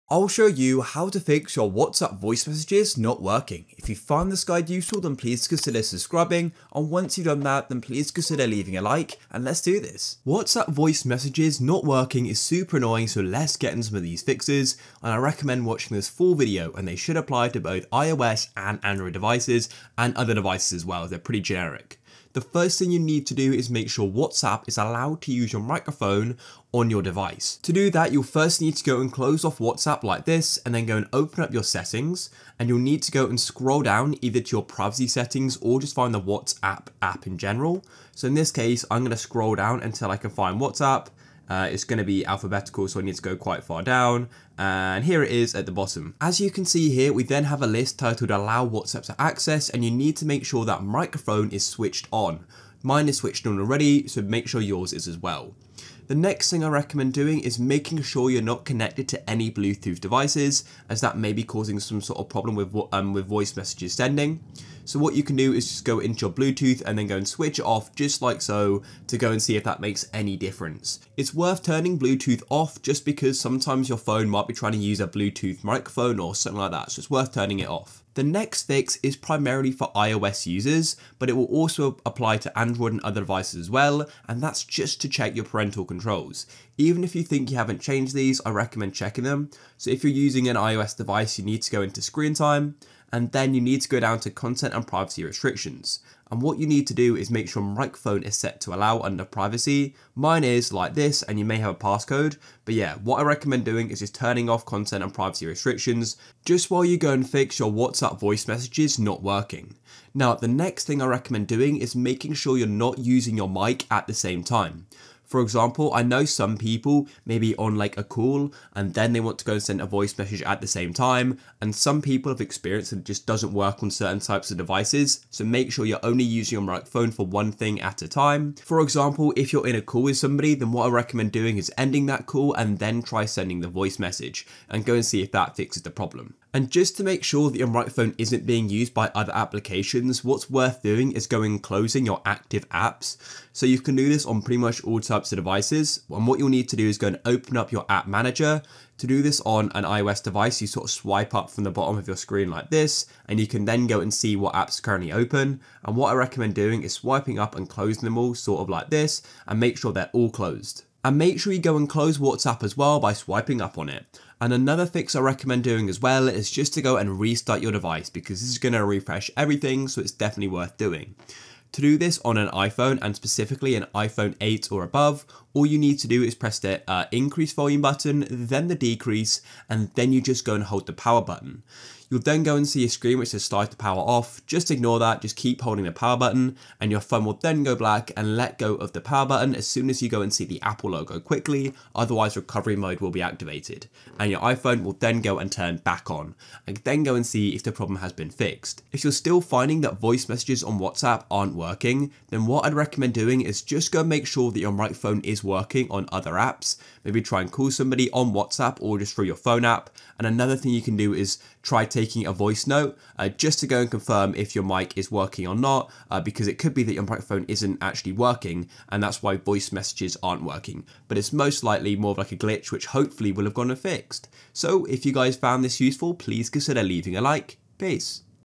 How To Fix WhatsApp Voice Message Problems - Full Guide
I show you how to fix whatsapp voice message problem in this video.

GuideRealm